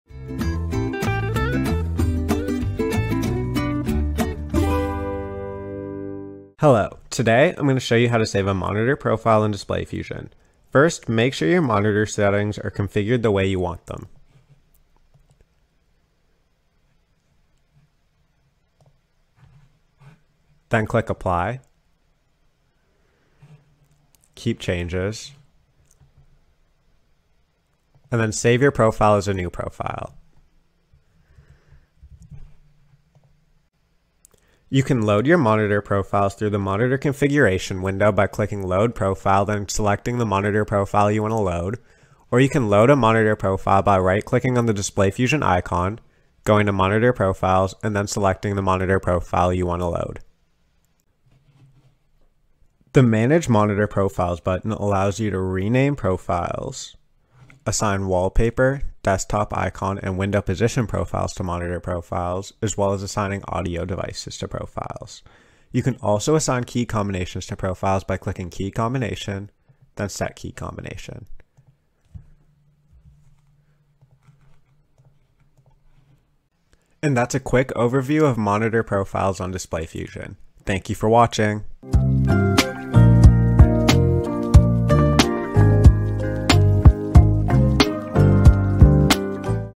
DisplayFusion: Monitor Profiles
Use DisplayFusion's powerful Monitor Profiles feature to create monitor configurations and easily load them when needed.

Try DisplayFusion for free today!